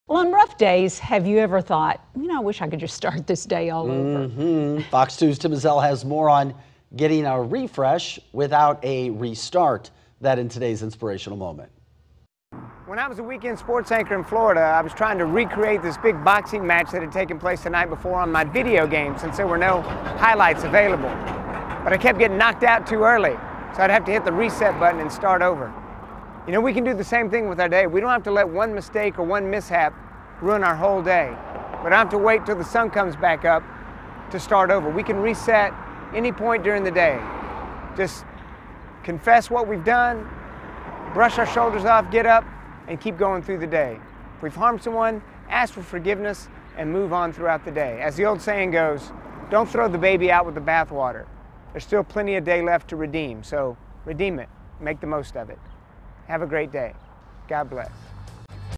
Tim Ezell's Inspirational Moments: Get a refresh without a restart
On rough days, have you ever thought: "I wish I could start this day over." Here's how to get a refresh without getting a restart.